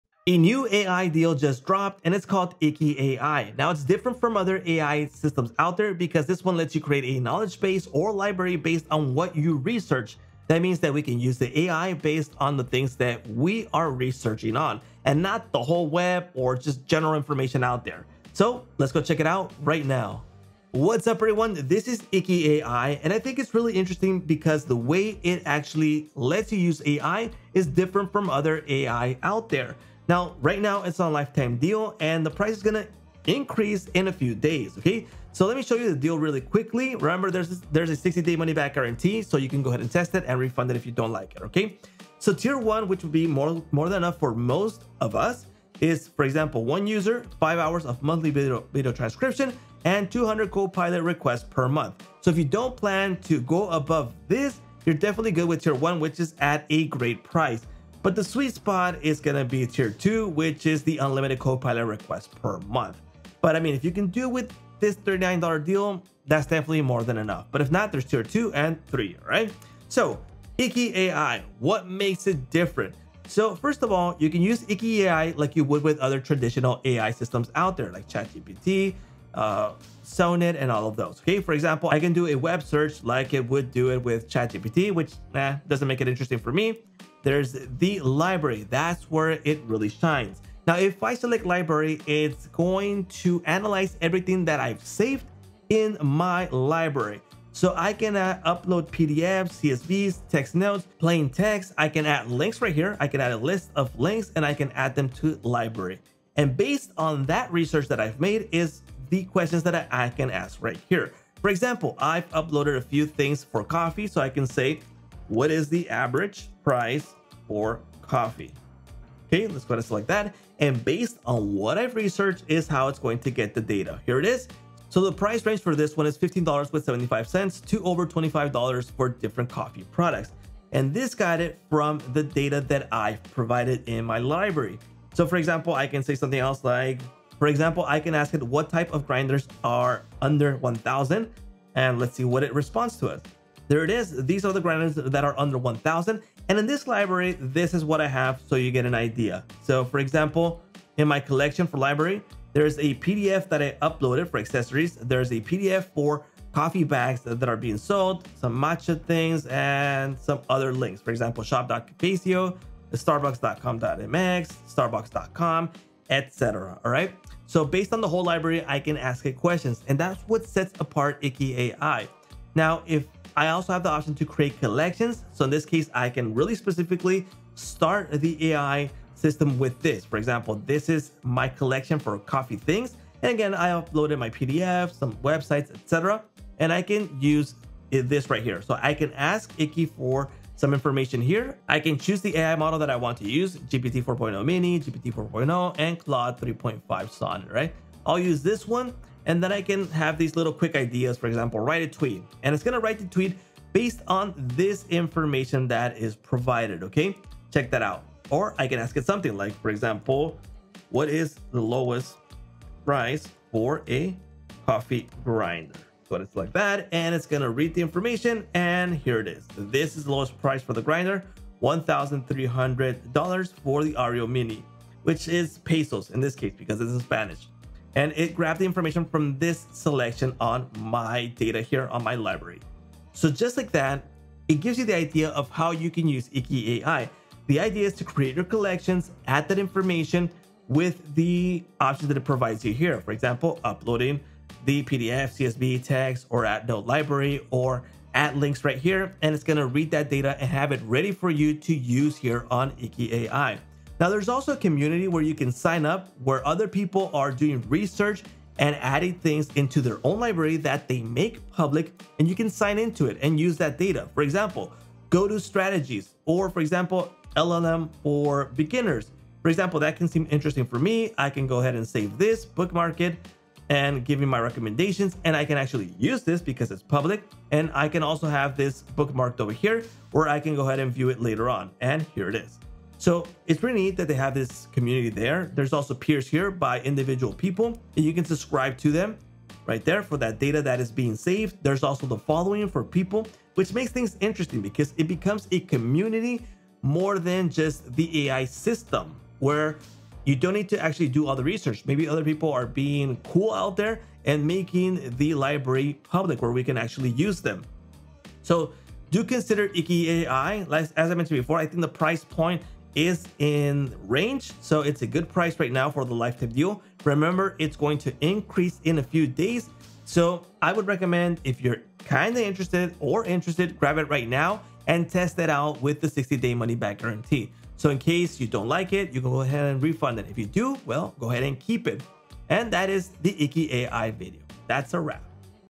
Why IKI.AI Library System Will Change Your Research - Appsumo Lifetime Deals
🔥 Don't Miss This IKI.AI Lifetime Deal! 🔥 Discover how IKI.AI transforms your research into a powerful digital library, with an AI copilot that’s unlike any other. Say goodbye to generic information overload and hello to a personalized knowledge base that evolves as you do. I'll demonstrate how you can fetch info, absorb knowledge, and brainstorm ideas all in one place. Imagine uploading everything from PDFs and Figma files to YouTube videos, and having your AI copilot ready to answer questions based on your unique library. This lifetime deal offers a variety of tiers, with options for unlimited co-pilot requests! With a 60-day money-back guarantee, there's no reason not to grab it now before the price increases. Plus, join a vibrant community where shared libraries can elevate your projects even further. Check out IKI.AI and transform the way you handle digital research today! 🚀

CHAPTERS:
00:00 - A New AI Deal Just Dropped
00:19 - IKI.AI Overview
01:09 - How IKI.AI Functions
04:15 - IKI.AI Community Insights